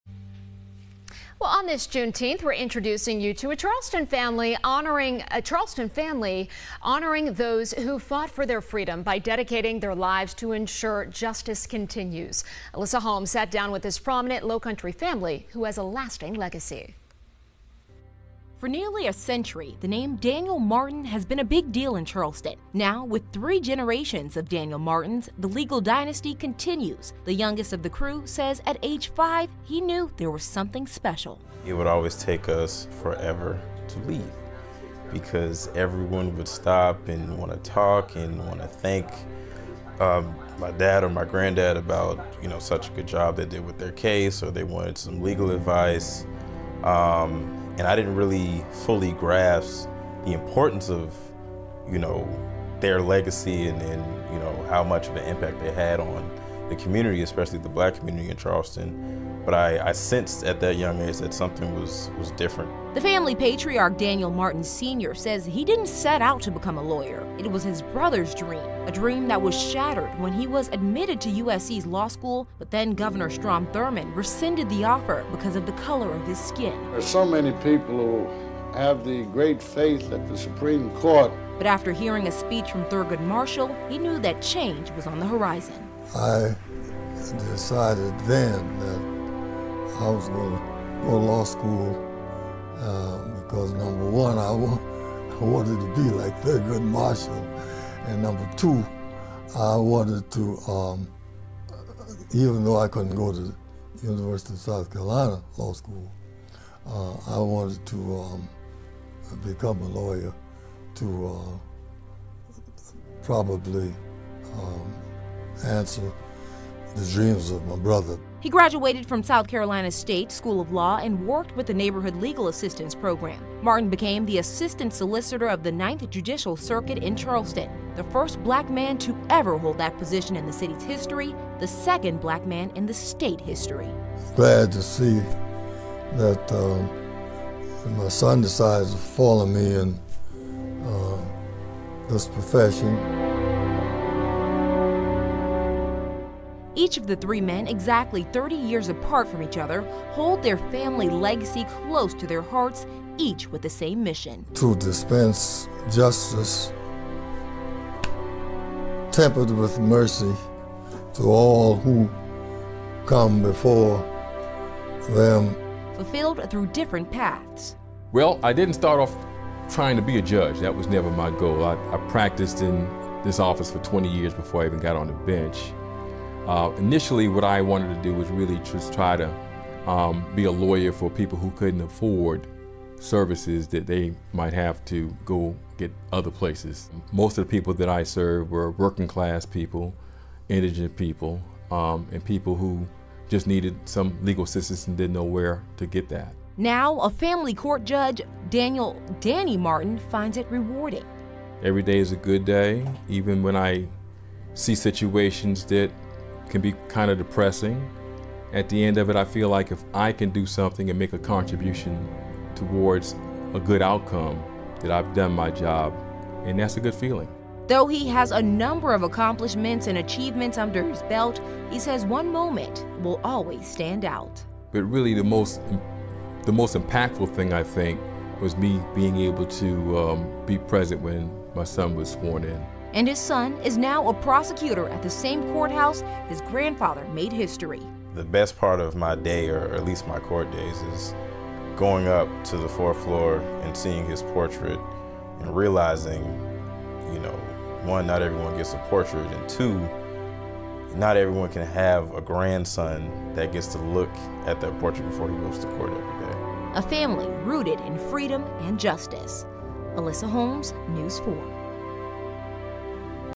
Charleston's three generations of Martins champion justice
For nearly a century, the name Daniel Martin has been prominent in Charleston. Now, with three generations of Daniel Martins,’ the legal dynasty continues.

The youngest of the crew, Daniel Martin III, says that by age 5, he knew their family was something special.

"It would always take us forever to leave because everyone would want to stop and talk and thank my dad or my granddad about such a good job they did with their case or some legal advice," Martin III said. "I didn't really fully grasp the importance of their legacy and how much of an impact they had on the community, especially the black community in Charleston. But I sensed at that young age that something was different.”